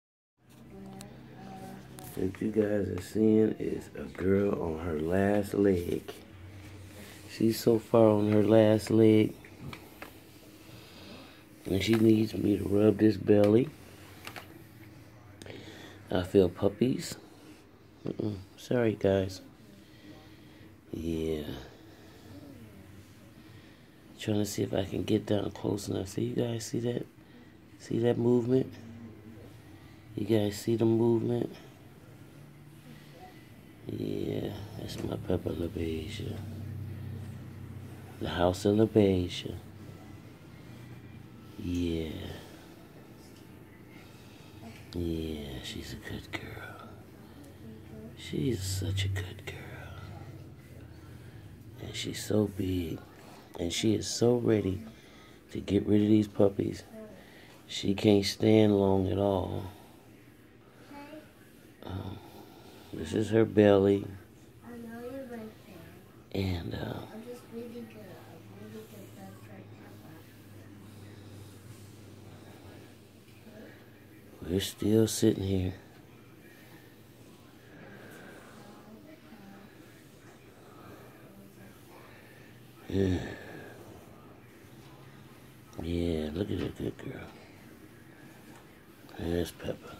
Zeus & Peppa LaBeija And They're Adventures Peppa's Last Few Days Of Puppy watch
Adventures Of My Two Corsos. ZEUS & Peppa

Zeus and Peppa LaBeija on FaceBook
zeusnpeppa @gmail.com